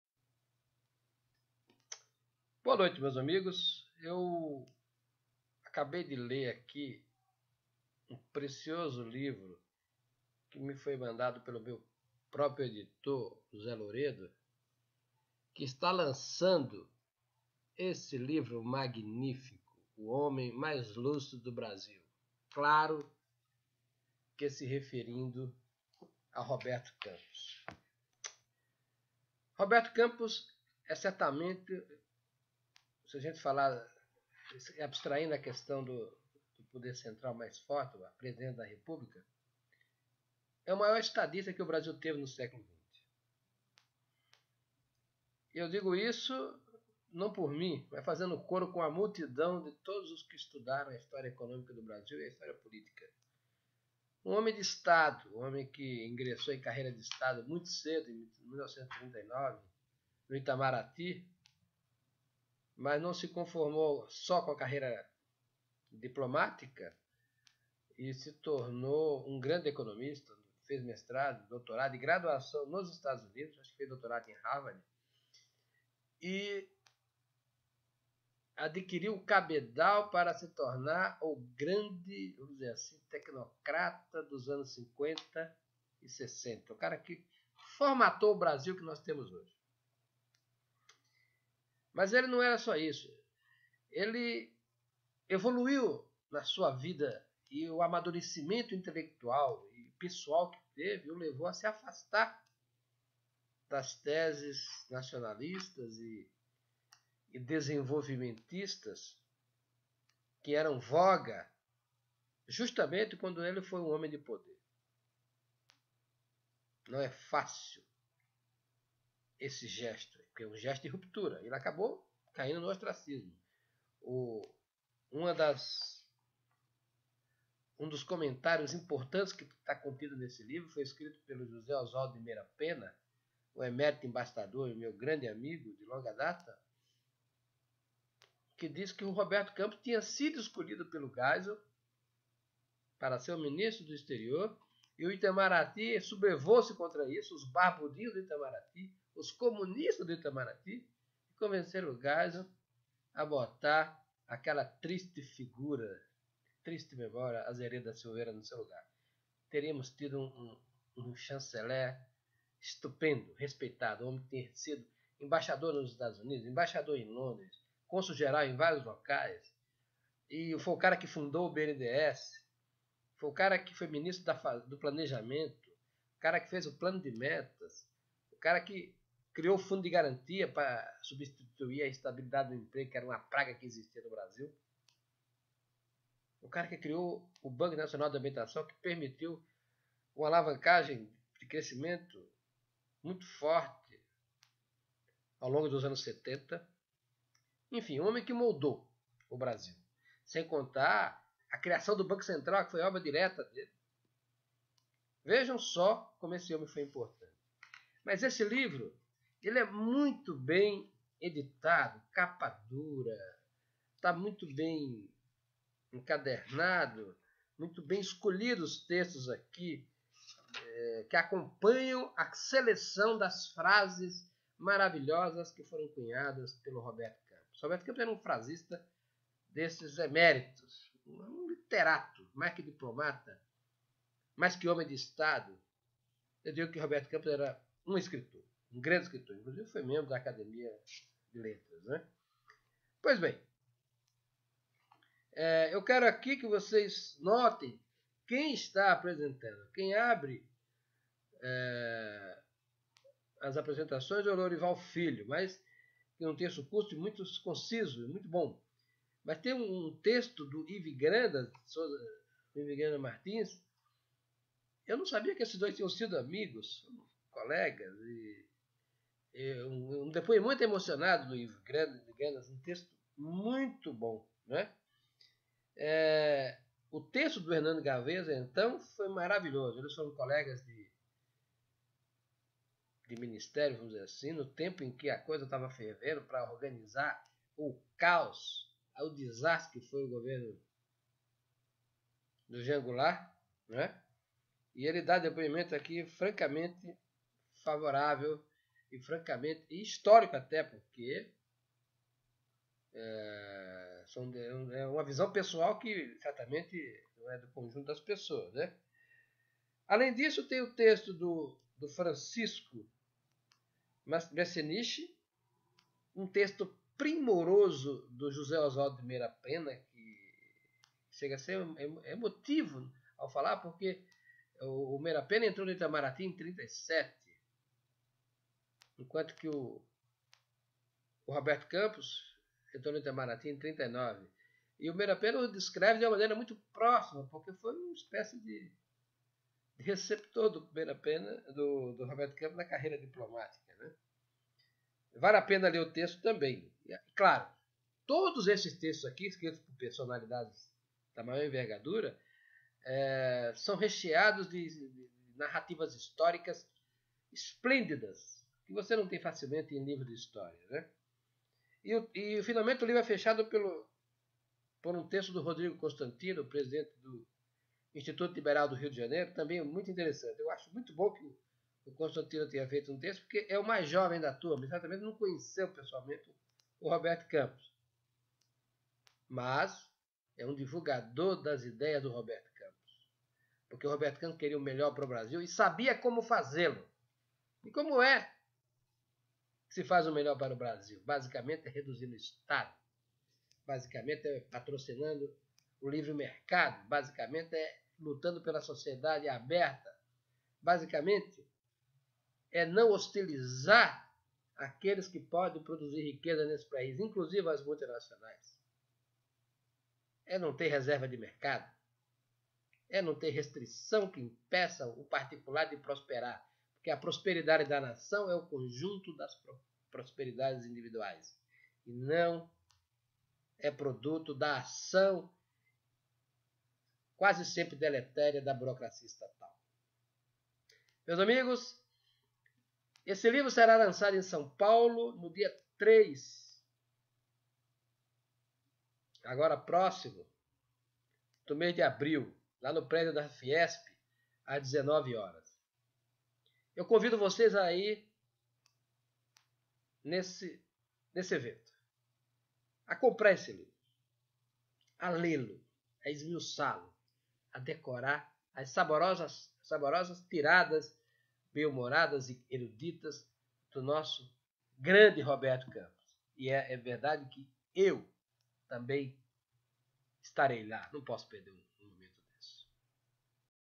O homem mais lúcido do Brasil
No próximo dia 03 de abril, às 19 horas, será lançado, no recinto da Fiesp, na Avenida Paulista, o livro O Homem Mais Lúcido do Brasil, as melhores frases de Roberto de Campos. Claro que eu estarei lá. Convido vocês a prestigiarem esse momento importante da nossa criação cultural.